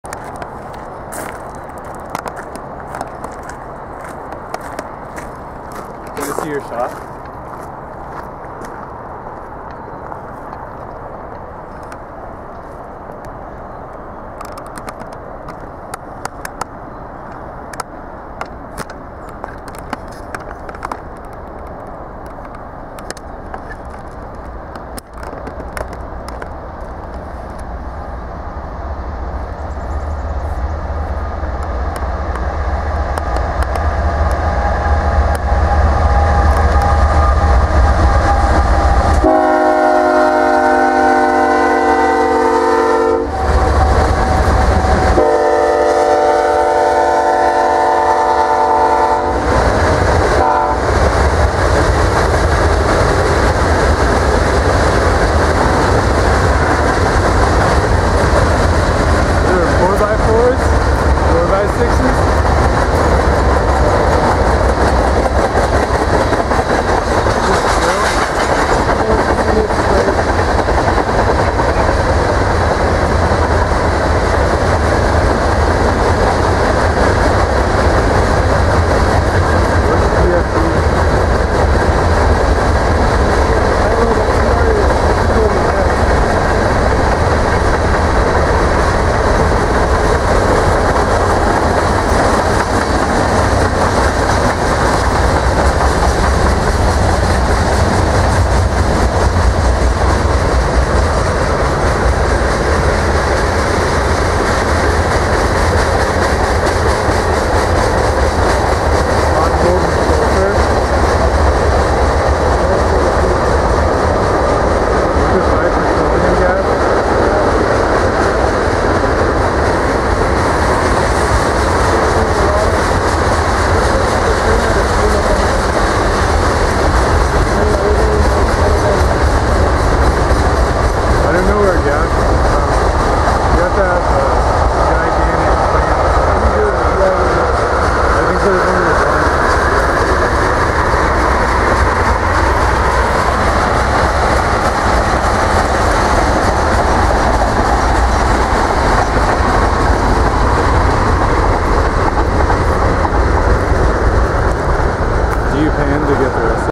SouthBound BNSF 7335 9-16-2015
Southbound BNSF 7335,8364 M-INB-ABL-16 Past Rocky Point On BNSF Seattle Sub Watch In 1080P